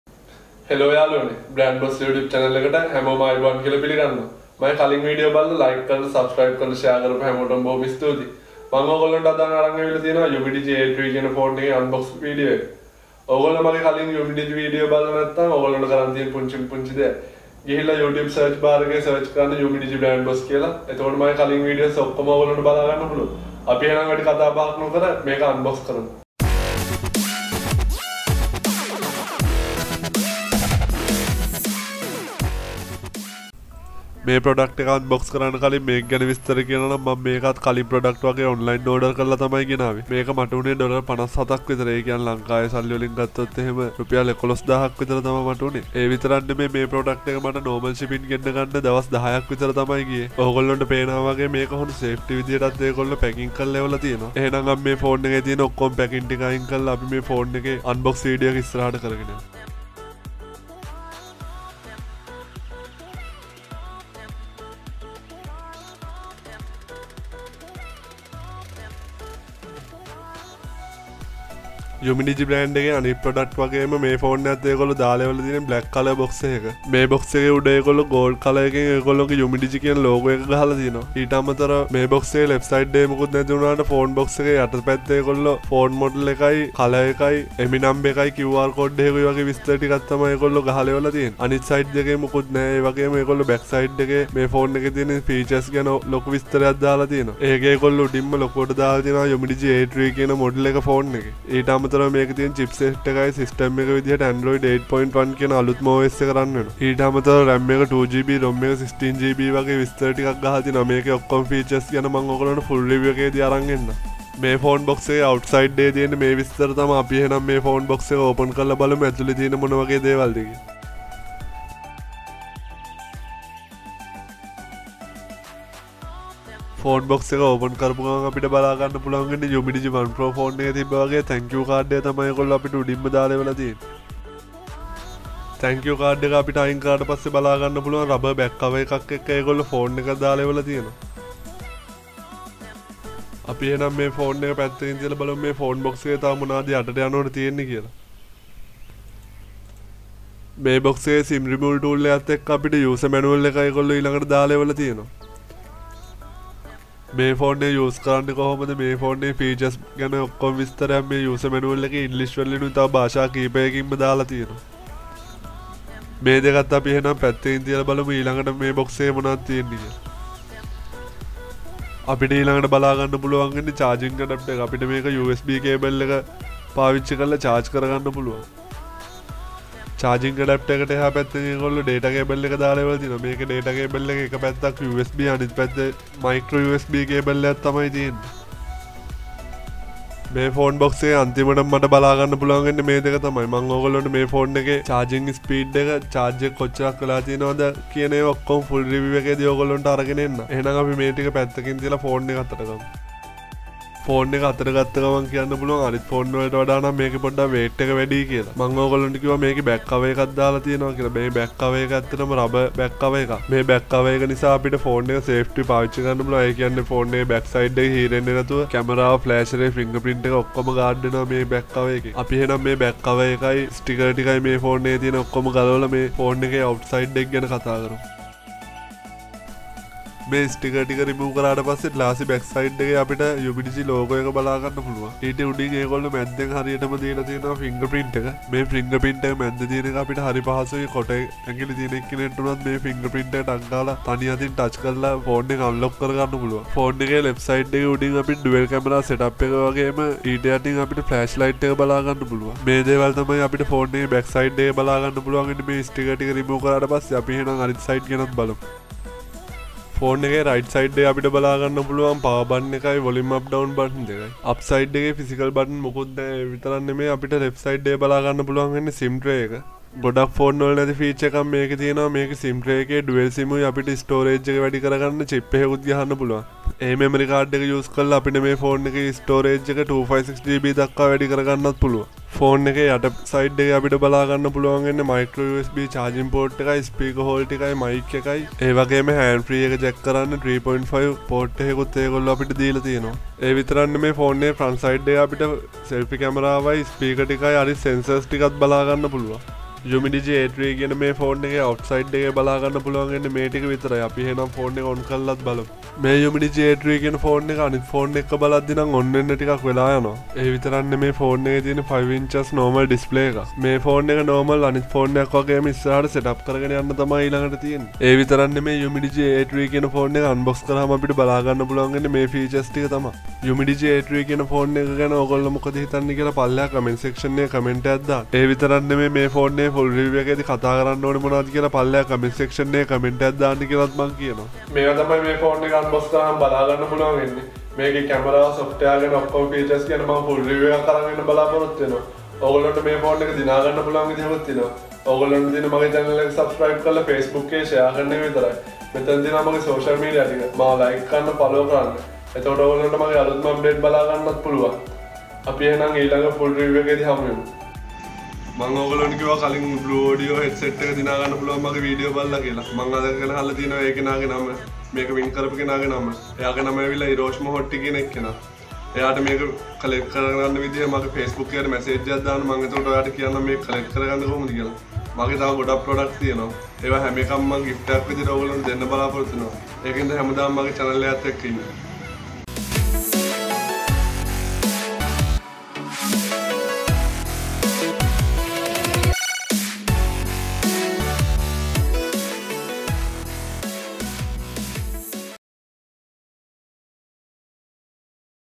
UMIDIGI A3 Unboxing & Quick Look | Best Budget Smart phone for $50
UMIDIGI A3 Unboxing සිංහලෙන්.

UMDIGI has launched the new A3 & A3 Pro, which has emerged as the best budget phone of this Q4 of 2018 with unbelievable specifications.  It has the dimensions of 147.19 x 70.2 x 9.5 mm making it the perfect fit in one’s hand. The 5.7-inch screen also makes it easy to handle with only one hand. The UMIDIGI A3 is equipped with an MTK6739 1.5GHz Quad Core, p Processor with Quad Core and 1.5GHz and 2GB of RAM with 16GB of storage, and 3300mAh battery. it have 12MP + 5MP dual-camera on the back and  8MP snapper with an identical aperture and supporting Beauty mode.  The device is also embedded with a Fingerprint Scanner.